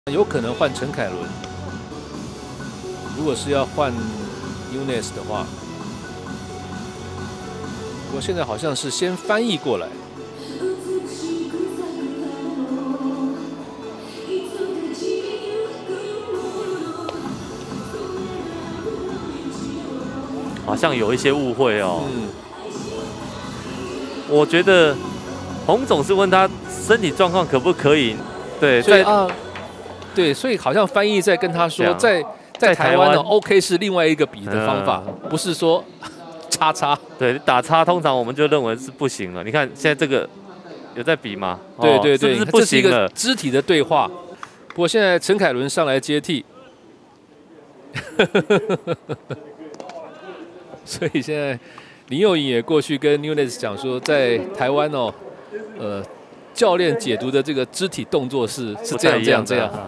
20210811｜不是英文不好！X肢體語言小插曲🤣
今天的八局上洪總跟紐那斯
因為肢體動作的場邊小插曲
洪總的Ｘ：還可以繼續嗎？不行嗎？
紐那斯的Ｘ：可以！不用換人

林老闆場邊解釋台版肢體動作：
「可以繼續 OK!」= 🙆‍♂️
「不行繼續 NO!」= 🙅‍♂️